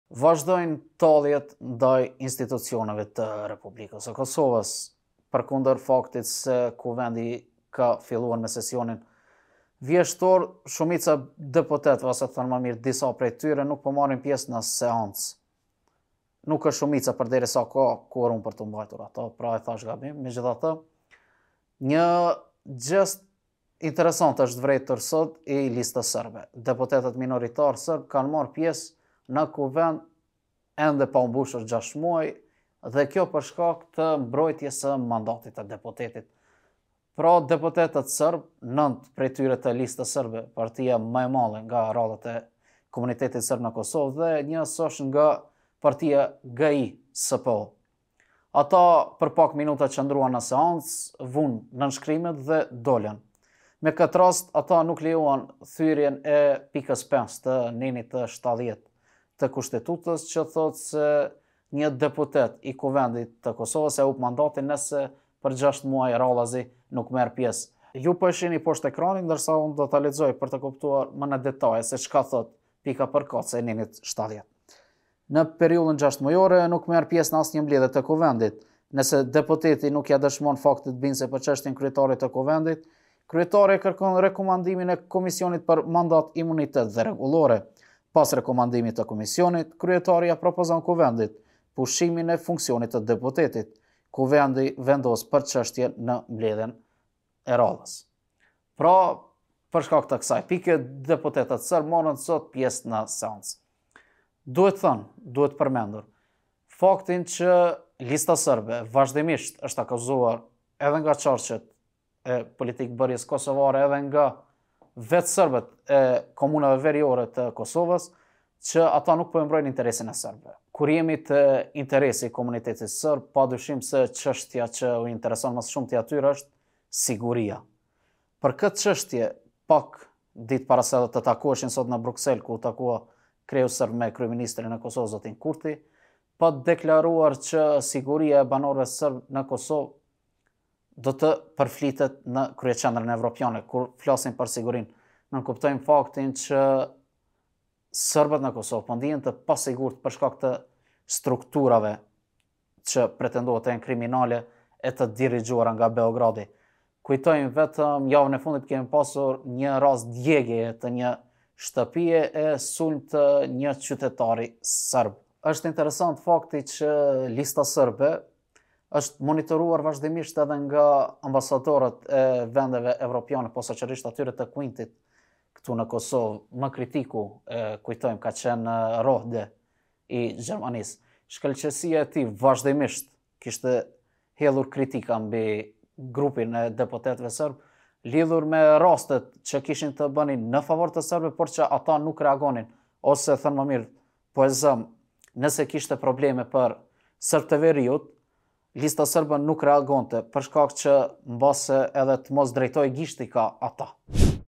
Lista Serbe me pak minuta në seancë “shpëtoi” mandatin
Çka thotë ligji: kur mund ta humbin mandatin deputetët serbë?